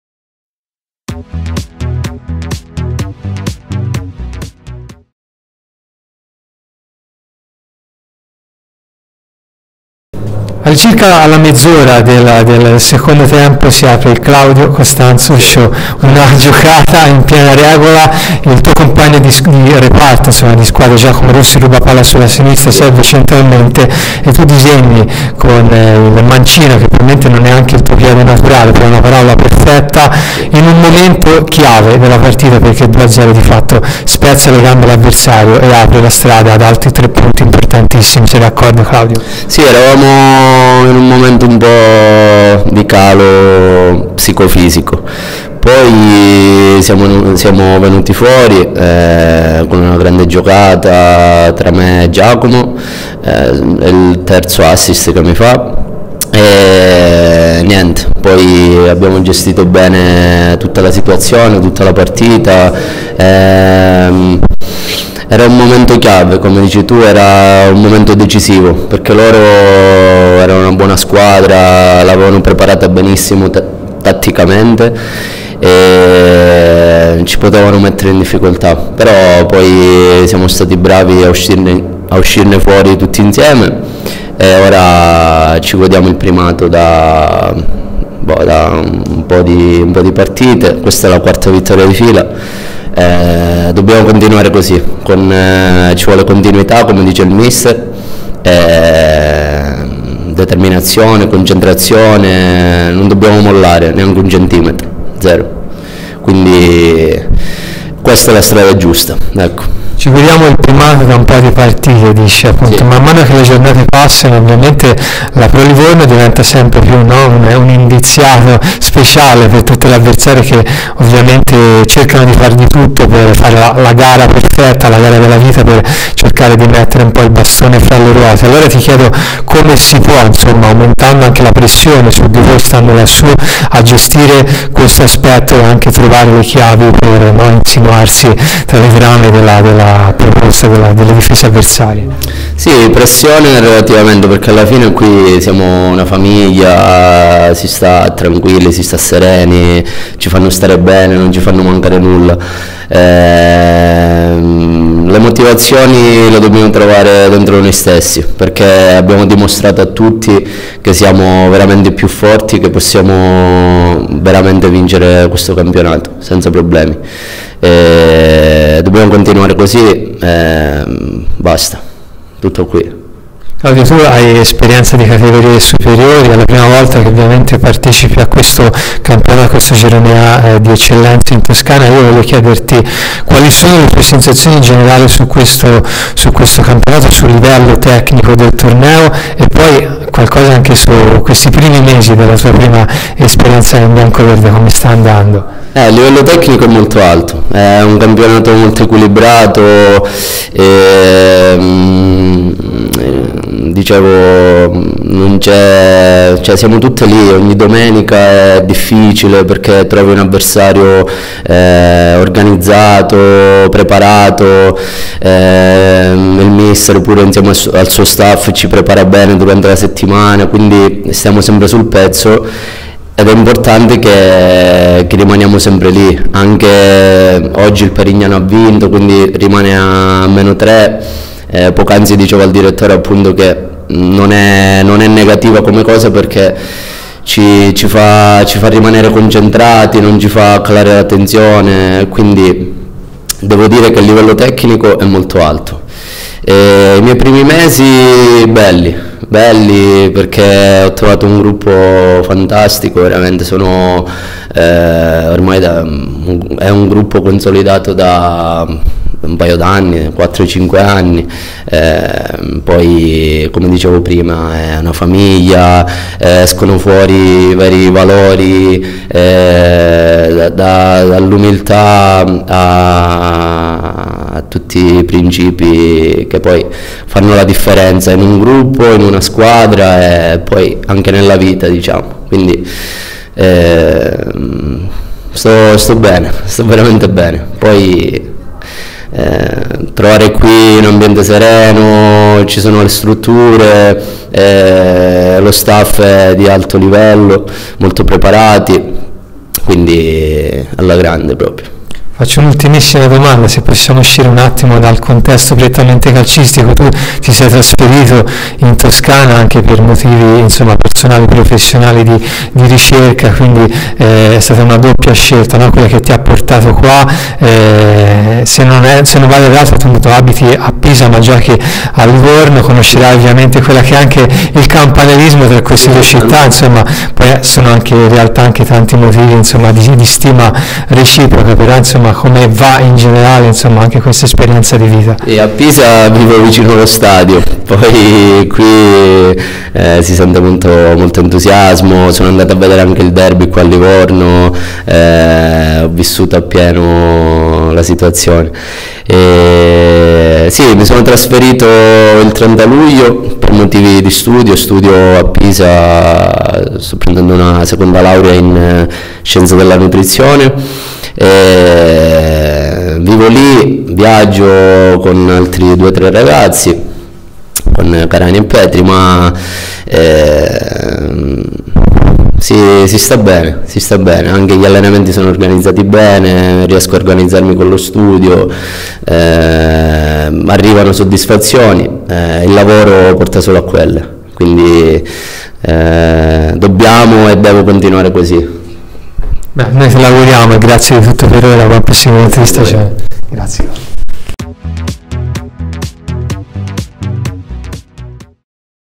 Pro Livorno Sorgenti - Castelfiorentino (1/12/19): intervista a Claudio Costanzo
L'intervista a Claudio Costanzo, centrocampista biancoverde, dopo la gara vinta dalla Pro Livorno Sorgenti per 2-0 contro il Castelfiorentino, nella tredicesima giornata del campionato di Eccellenza girone A, giocata al Magnozzi domenica 1 dicembre 2019.

PRO LIVORNO SORGENTI – CASTELFIORENTINO 2-0
PLS: Blundo, Solimano (80’ Grossi), Petri, Carani, Salemmo, Filippi, Brizzi (46’ Bulli), Costanzo, Granito, Matteoli (46’ Angiolini), Rossi. A disp.: Vozza, Del Corona, Bartorelli, Gazzanelli, Milano, Casalini.  All.: Niccolai.
CASTELFIORENTINO: Chiarugi, Pellegri (46’ Sarti N.), Vecchiarelli, Olivieri, Campatelli, Bagnoli (69’ Fossati), Montecalvo, Anichini (86’ Cioni), Pecchioli, Nidiaci (69’ Adragnia), Zizzari. A disp.: Orsini, Sarti M., Zazaj, Bindocci, Pieroni. All.: Ebeyer.
ARBITRO: Carsenzuola di Legnano.
RETI: 41’ Petri, 76’ Costanzo.
NOTE: ammoniti Campatelli, Pecchioli, Salemmo, Bulli, Sarti Niccolò, Angiolini.